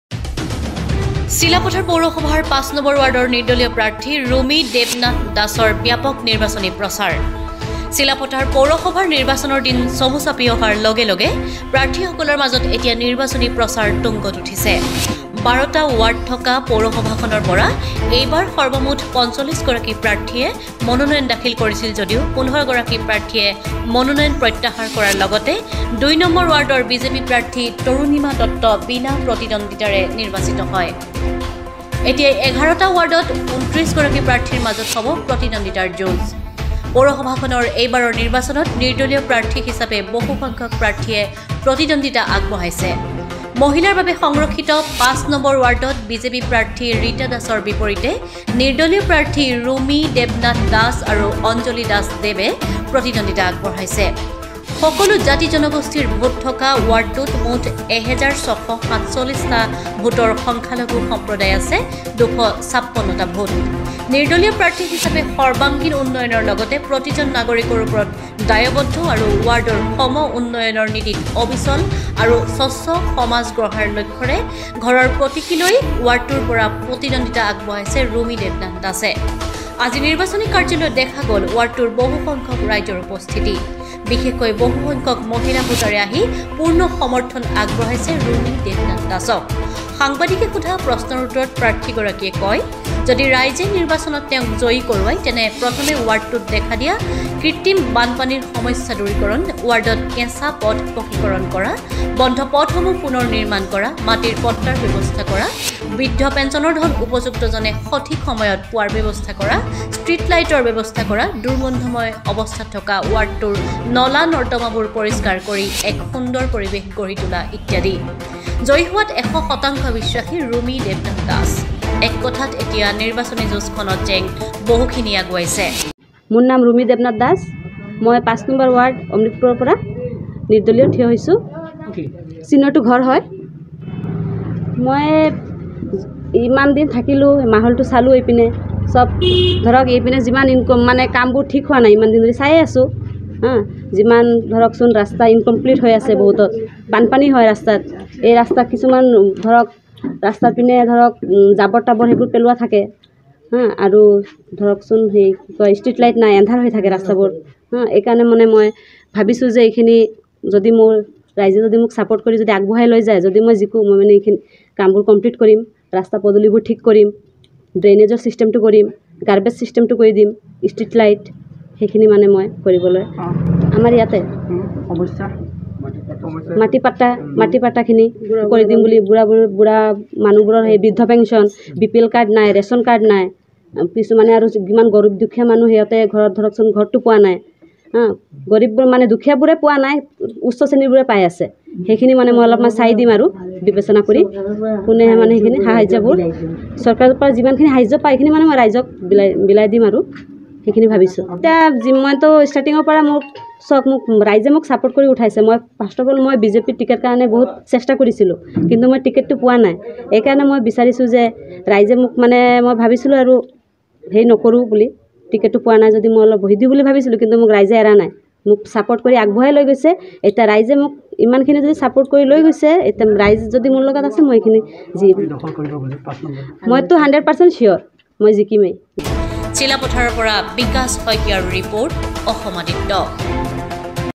চিলাপথাৰ পৌৰসভাৰ ৫ নং ৱাৰ্ডৰ নিৰ্দলীয় প্ৰাৰ্থী ৰুমী দেৱনাথ দাসৰ ব্যাপক নিৰ্বাচনী প্ৰচাৰ
চিলাপথাৰ পৌৰসভাৰ নিৰ্বাচনৰ দিন চমু চাপি অহাৰ লগে লগে প্ৰাৰ্থী সকলৰ মাজত এতিয়া নিৰ্বাচনী প্ৰচাৰ তুংগত উঠিছে । ১২ টা ৱাৰ্ড থকা পৌৰসভাখনৰ পৰা এইবাৰ  সৰ্বমুঠ ৪৫ গৰাকী প্ৰাৰ্থীয়ে মনোনয়ন দাখিল কৰিছিল যদিও ১৫ গৰাকী প্ৰাৰ্থীয়ে মনোনয়ন প্ৰত্যাহাৰ কৰাৰ লগতে ২ নং ৱাৰ্ডৰ বিজেপি প্ৰাৰ্থী তৰুনীমা দত্ত বিনা প্ৰতিদ্বন্দিতাৰে নিৰ্বাচিত হয় ।